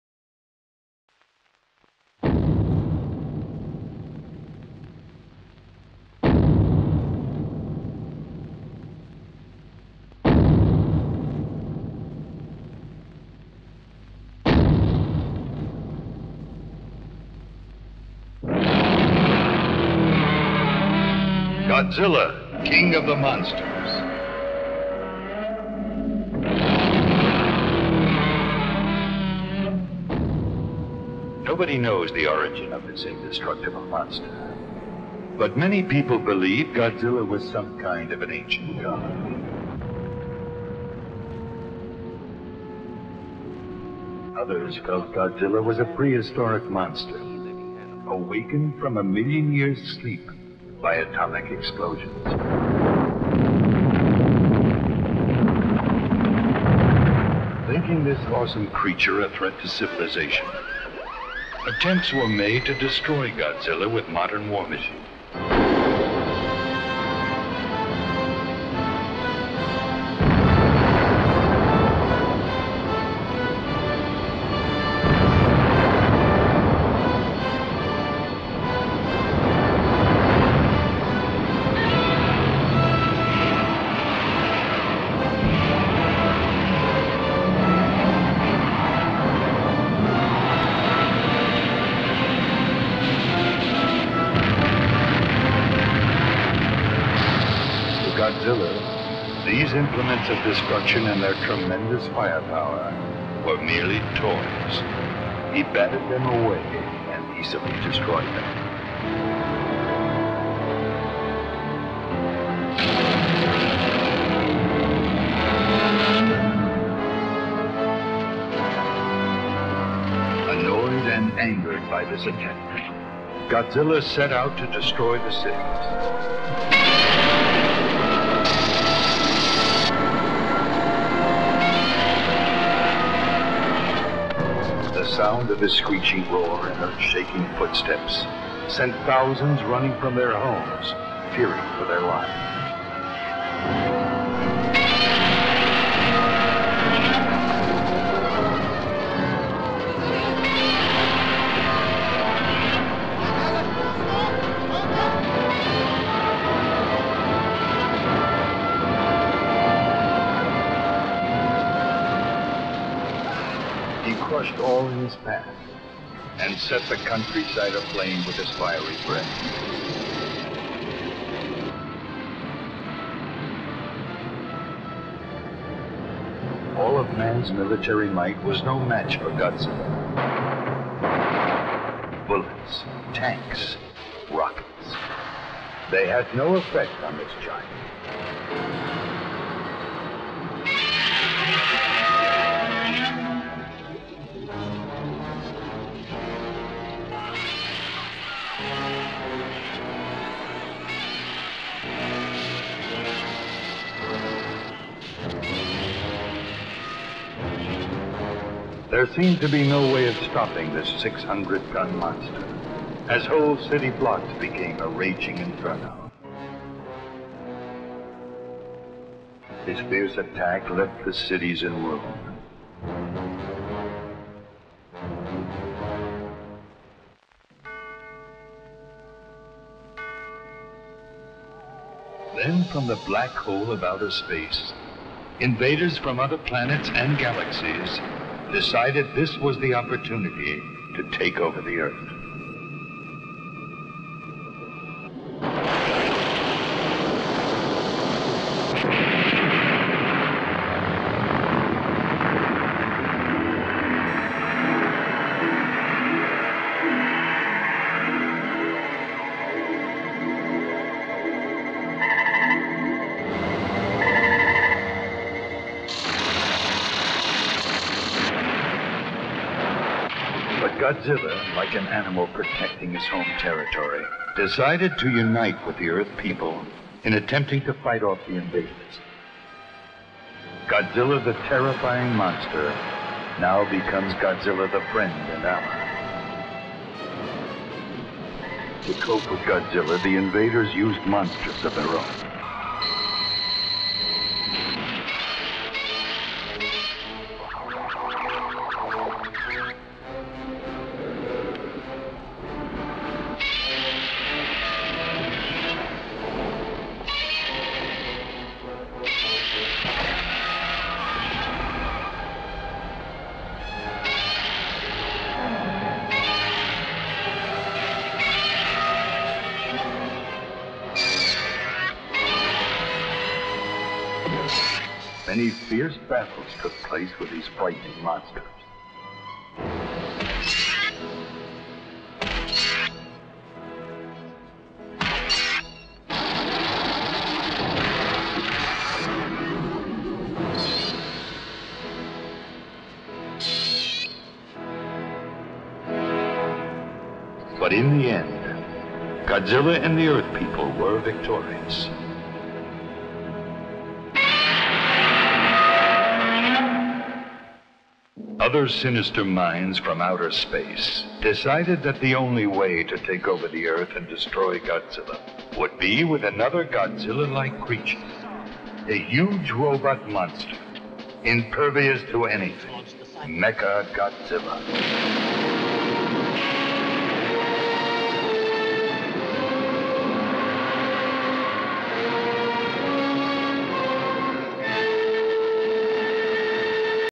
The Legend of Godzilla
How Godzilla, the terrifying monster, became Godzilla, the friend and ally.